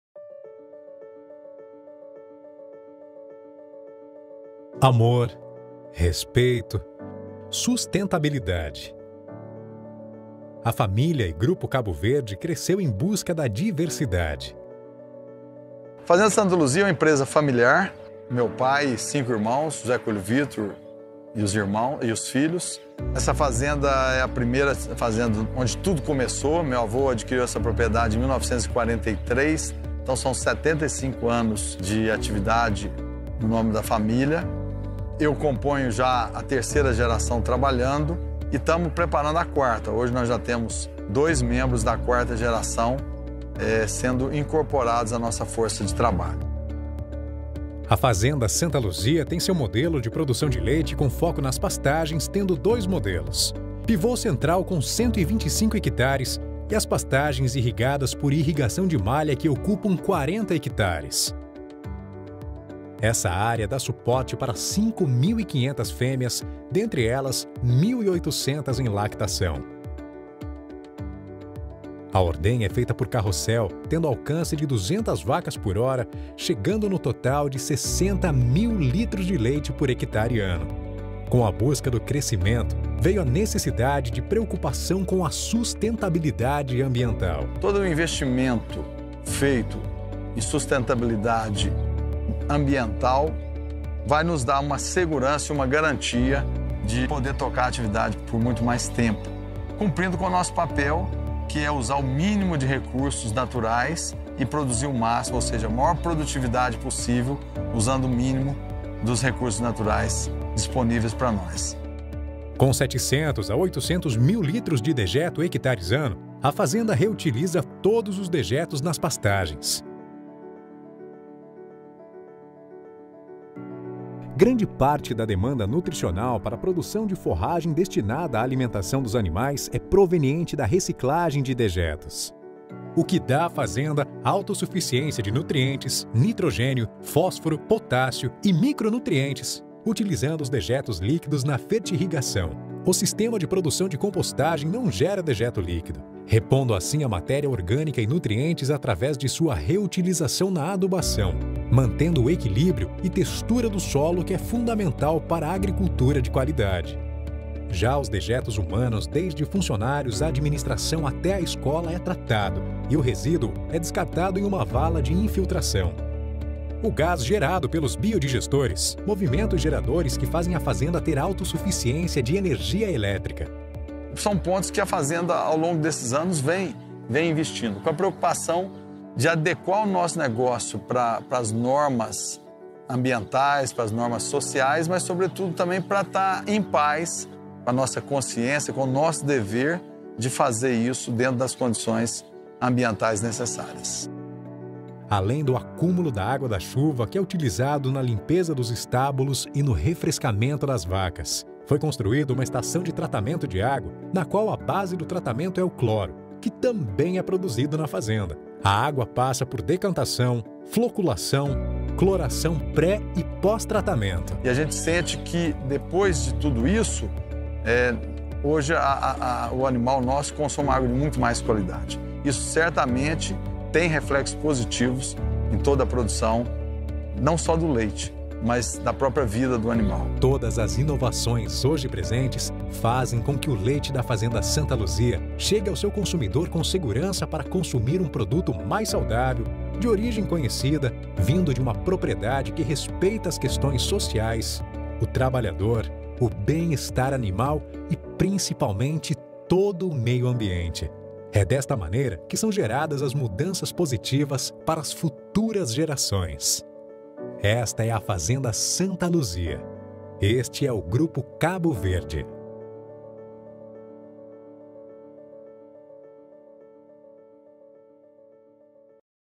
Fazenda Santa Luzia - Fatores Socioambientais (por Agroceres Multimix)
Confira o primeiro artigo da série, com vídeo, sobre a Fazenda Santa Luzia, produzida pela Agroceres Multimix. A fazenda, pertencente ao Grupo Cabo Verde, localizada em Passos-MG, é modelo vivo de gestão socioambiental dos recursos. Pensando em intensificar sua escala de produção de leite, o administrador da fazenda, Maurício Silveira Coelho, atentou-se aos mínimos detalhes e investiu em ferramentas capazes de trazer benefícios para seus negócios, para os funcionários e o consumidor final. A fazenda Santa Luzia e a Agroceres Multimix são clientes e parceiras do Ideagri.